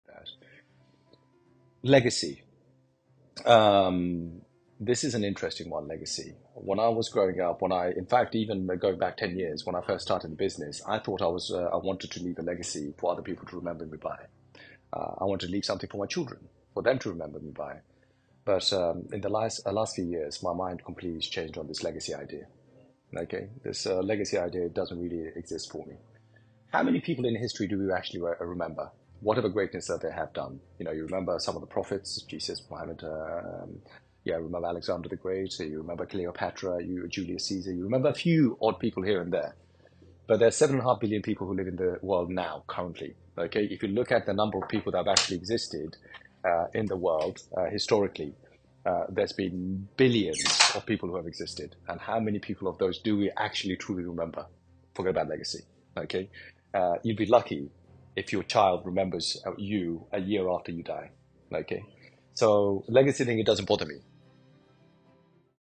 Leaving a Legacy: How My Mindset Changed Over Time
In the last few years, my perspective on legacy has changed - what was once a desire to be remembered by has shifted to one of striving to leave something valuable for my children. It's not about being remembered, it's about making an impact.